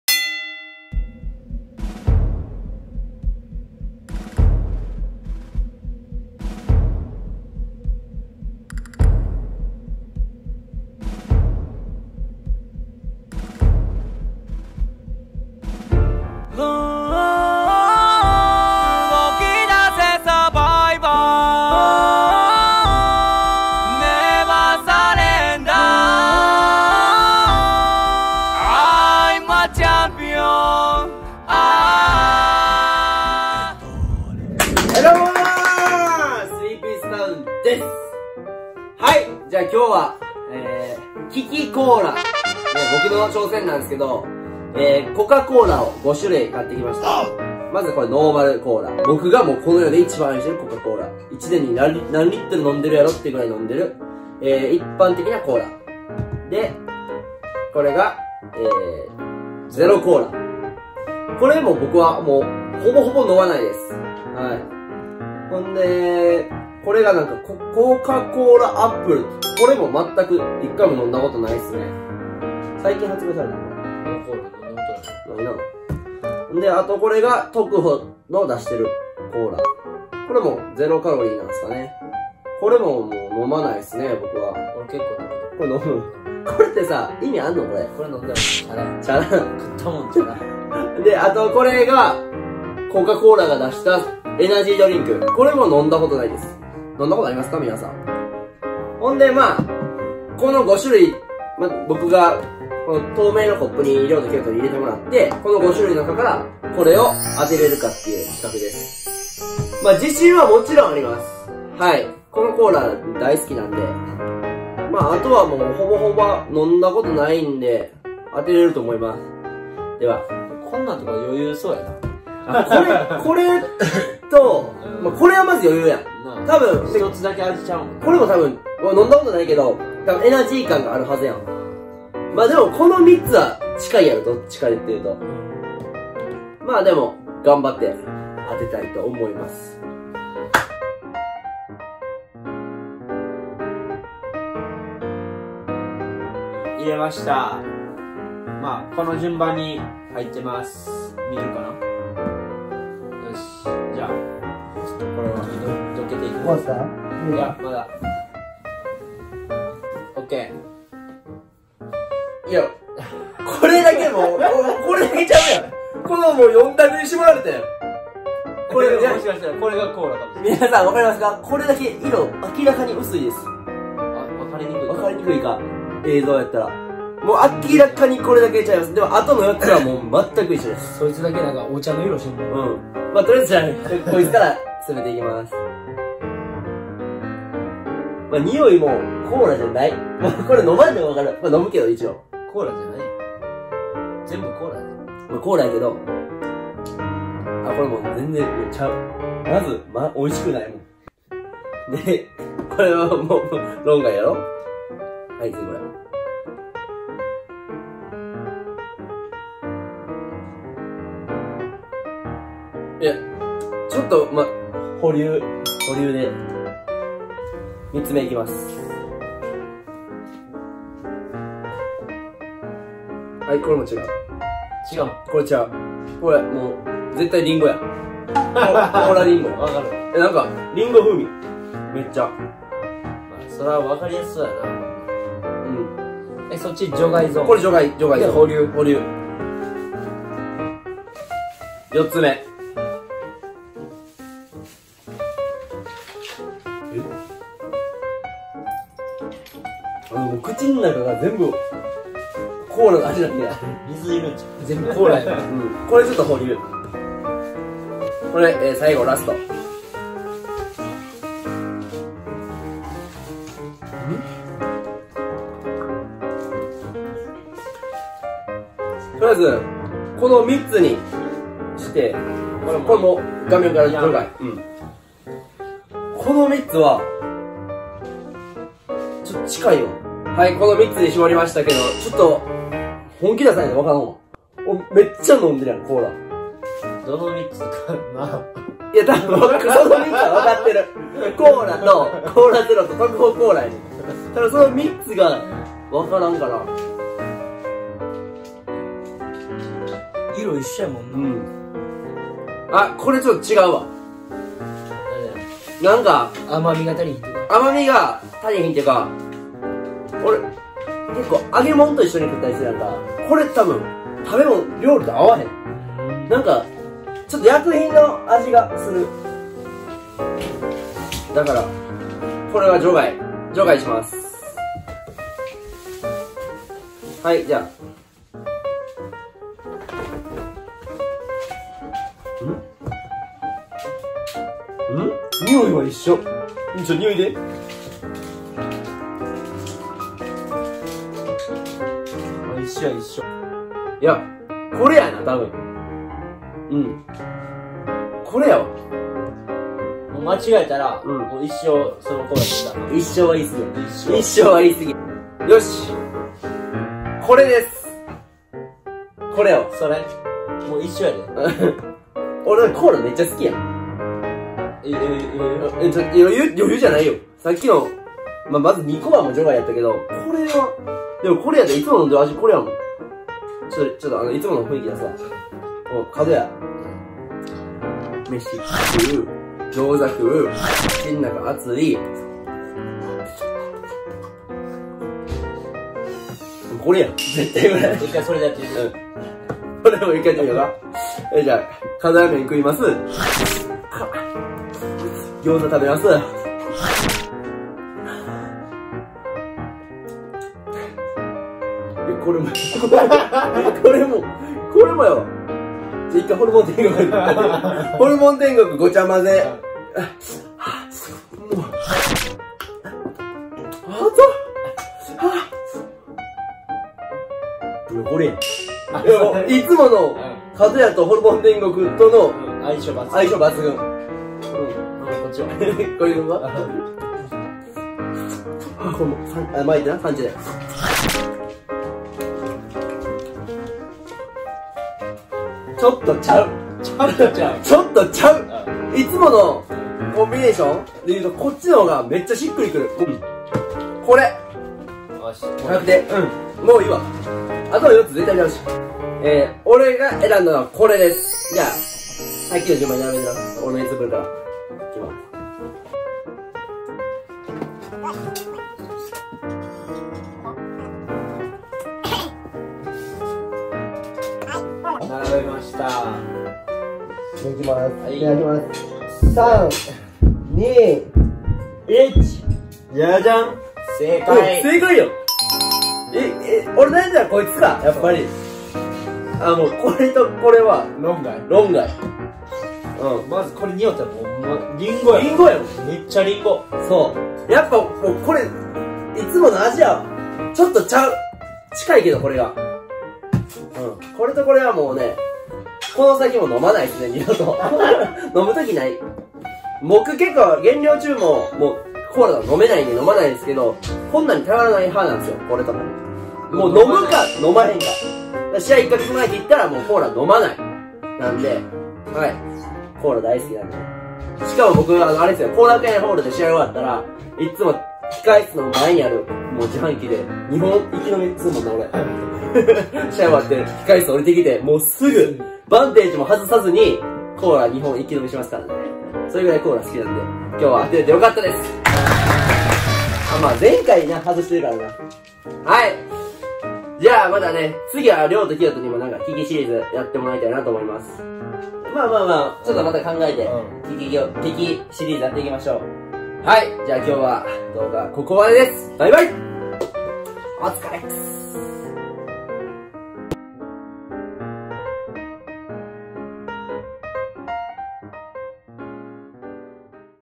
【利きシリーズ】世界１コカコーラを愛する長男の挑戦
利きシリーズ第２弾、今回はコカコーラで挑戦！

【試合情報】
１２月１４日（土）　東京後楽園ホール
NKB主催　Japan Sift Land presents
５９kgトーナメント
髙橋　亮　出場
皆さま亮の応援よろしくお願いします！

DMにて連絡よろしくお願いします！